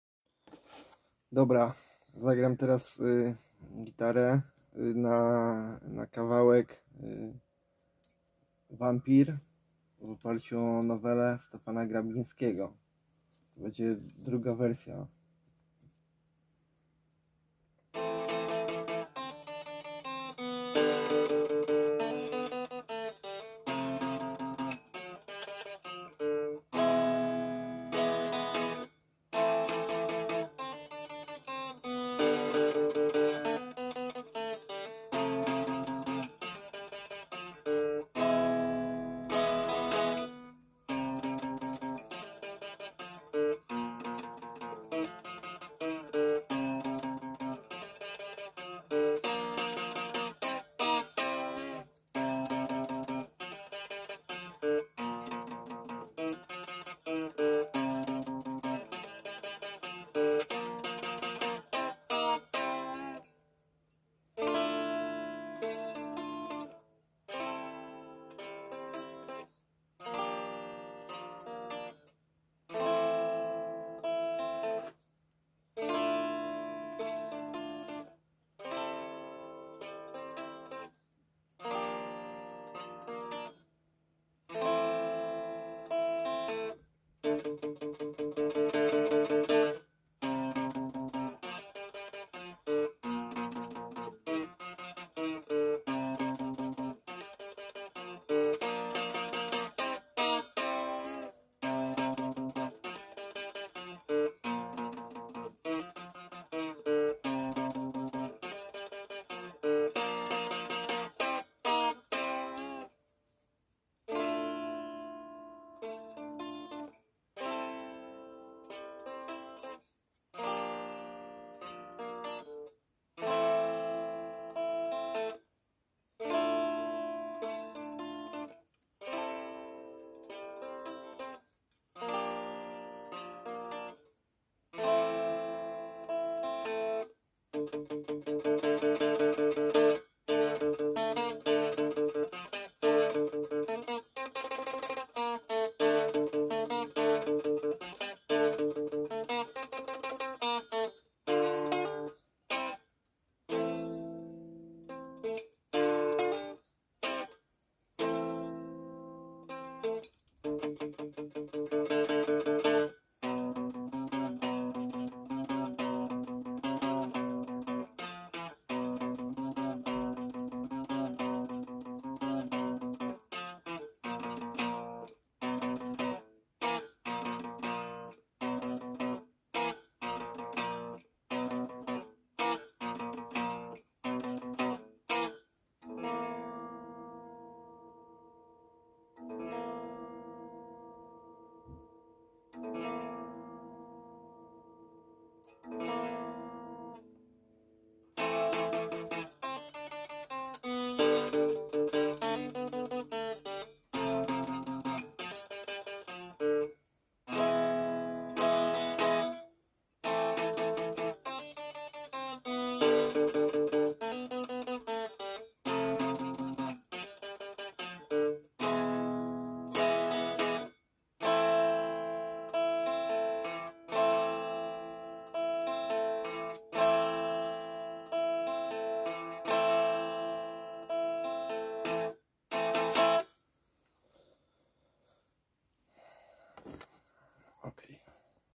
Raban z Gitarą ( wersja surówka )
W Wigilię sobie zacząłem plumkać melodyjki i tym sposobem wymyśliłem piosenkę . Mam już nagraną z drugą partią gitary w tle . Oraz jakoś połową sekwencji na basówce.oraz wstępną perkusją.Kombinowałem , Myślałem,zastanawiałem się nad koncepcją .Może będzie to z tekstem a może wersja bez czyli instrumentalna.